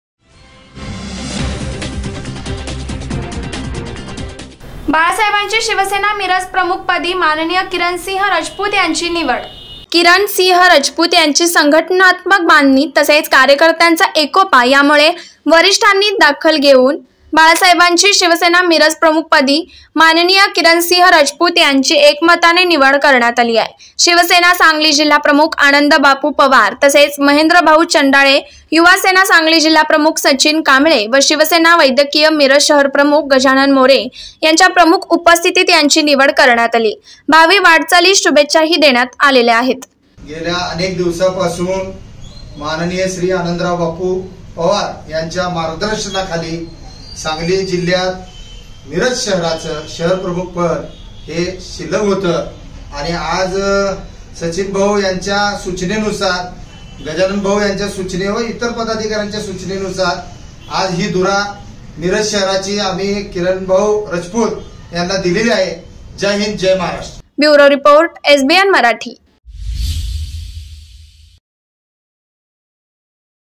SANGLI | बाळासाहेबांची शिवसेना मिरज प्रमुख पदी मा किरणसिंह राजपूत यांची निवड
बाळासाहेबांची शिवसेना मिरज प्रमुख पदी मा किरणसिंह राजपूत यांची निवड